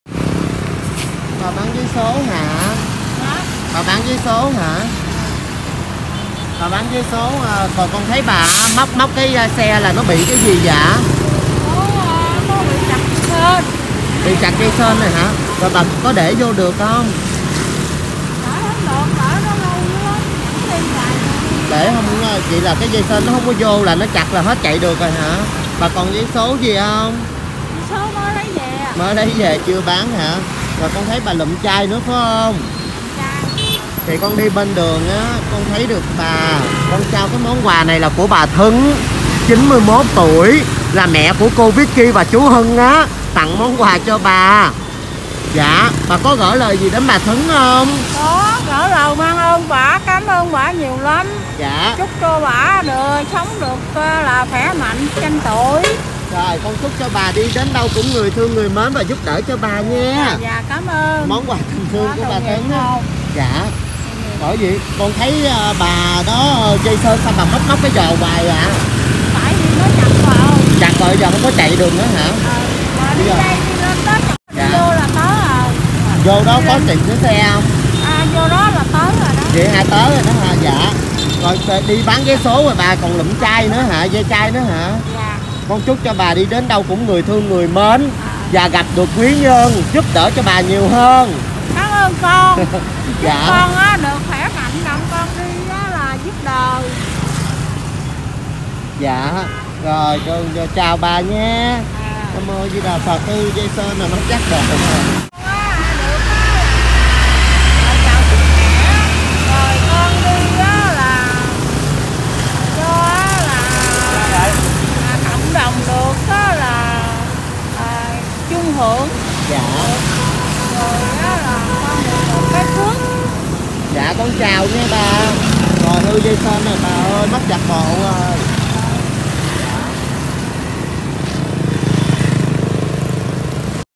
Tập 44__trời mưa lớn mới vừatạnh têtái cả lòng chăng dáng hình bà cụ lômkhơm hư dây sên chiếc xe đạp
trời mưa lớn mới vừatạnh têtái cả lòng chăng dáng hình bà cụ lômkhơm hư dây sên chiếc xe đạp